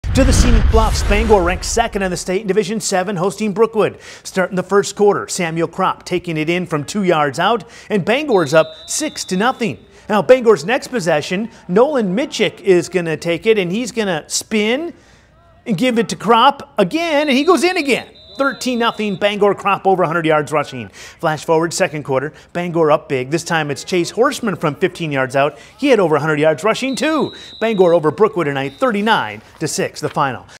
Bangor blows by Brookwood
Bangor turned to their patented rushing attack to lead them to a 39-6 win over Brookwood in a Scenic Bluffs showdown.